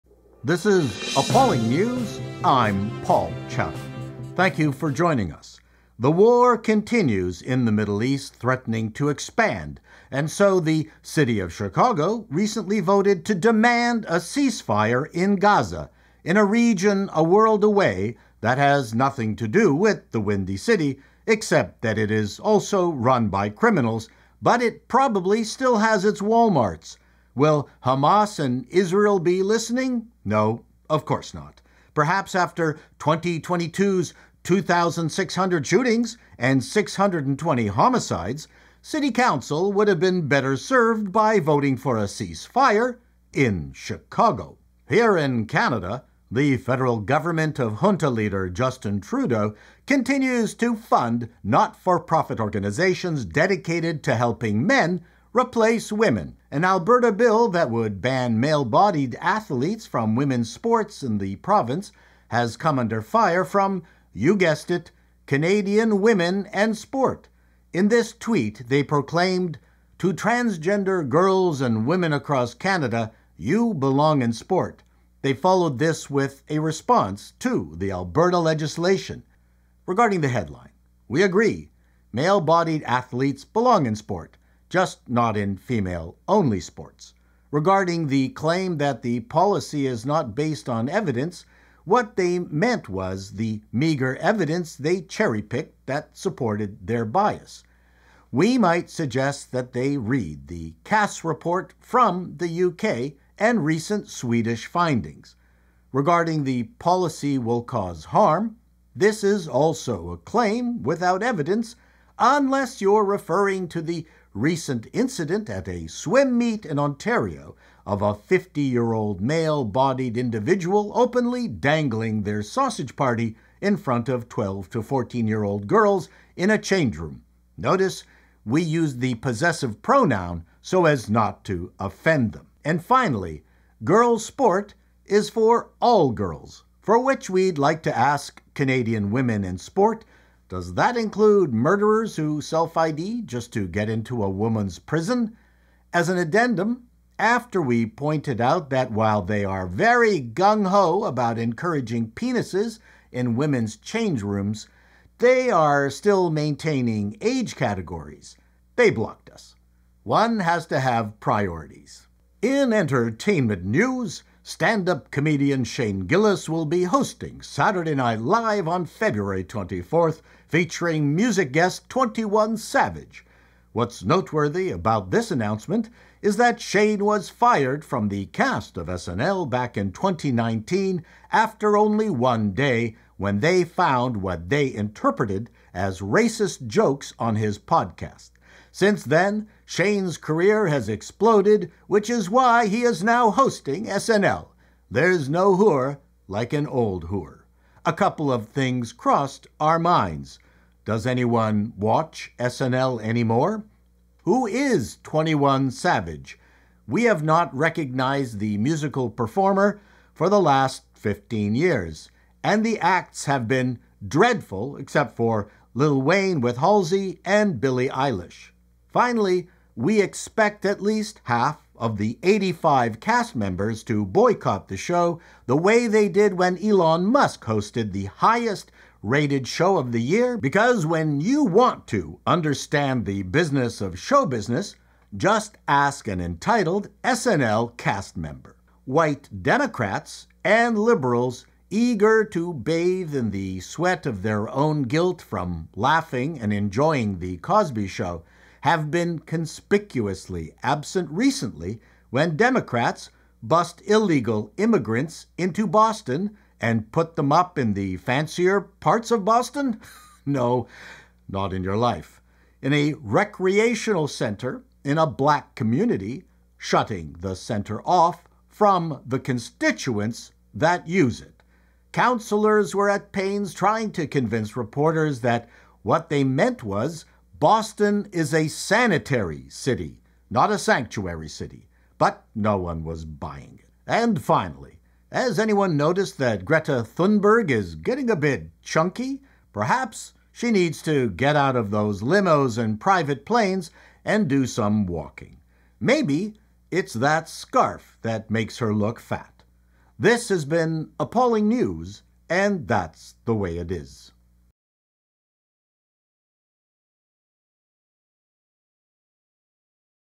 Chicago votes Hamas? Boston gets migrants? Women sports hate women? Greta regreta?
Just stuff I find amusing and horrific